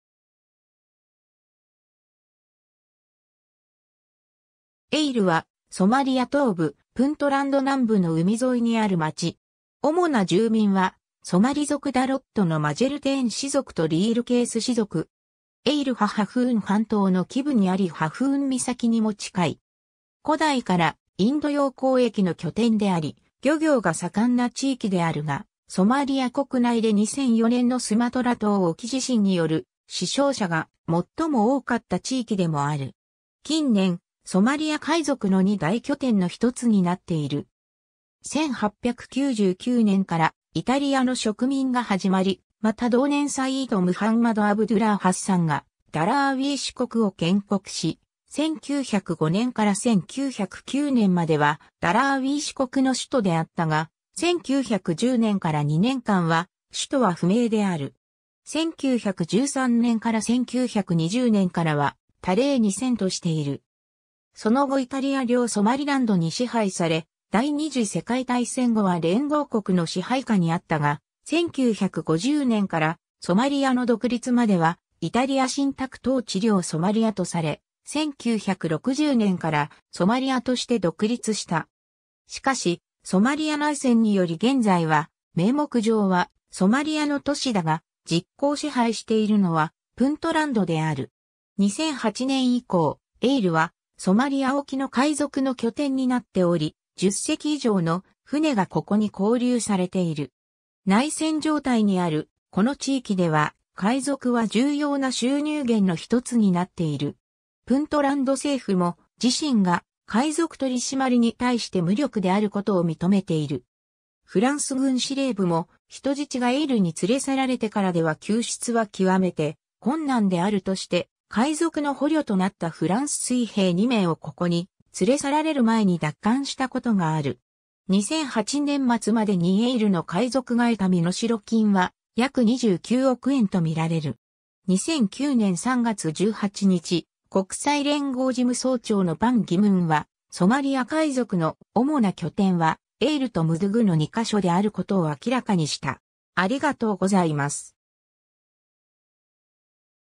エイル (ソマリア)
エイル（またはイール, 英: Eyl）はソマリア東部、プントランド南部の海沿いにある町。
主な住民はソマリ族ダロッドのマジェルテーン氏族とリールケース氏族 (Leelkase) 。
 エイルはハフーン半島の基部にありハフーン岬 (Hafun) にも近い。
古代からインド洋交易の拠点であり、漁業が盛んな地域であるが、ソマリア国内で2004年のスマトラ島沖地震による死傷者が最も多かった地域でもある。
近年、ソマリア海賊の2大拠点のひとつになっている。
 1899年からイタリアの植民が始まり、また同年サイイド･ムハンマド･アブドゥラー･ハッサンがダラーウィーシュ国を建国し、1905年から1909年までは、ダラーウィーシュ国の首都であったが、1910年から２年間は首都は不明である。
1913年から1920年からは、タレーに遷都している。
その後イタリア領ソマリランドに支配され、第二次世界大戦後は連合国の支配下にあったが、1950年からソマリアの独立まではイタリア信託統治領ソマリアとされ、1960年からソマリアとして独立した。
しかし、ソマリア内戦により現在は名目上はソマリアの都市だが、実効支配しているのはプントランドである。
 2008年以降、エイルはソマリア沖の海賊の拠点 (Pirate utopia) になっており、10隻以上の船がここに拘留されている。
内戦状態にあるこの地域では海賊は重要な収入源のひとつになっている。
プントランド政府も、自身が海賊取締りに対して無力であることを認めている。
フランス軍司令部も、人質がエイルに連れ去られてからでは救出は極めて困難であるとして、海賊の捕虜となったフランス水兵2名をここに連れ去られる前に奪還したことがある。
2008年末までにエイルの海賊が得た身代金は約29億円と見られる。
 2009年3月18日、国際連合事務総長のパン・ギムンは、ソマリア海賊の主な拠点はエイルとムドゥグの2箇所であることを明らかにした。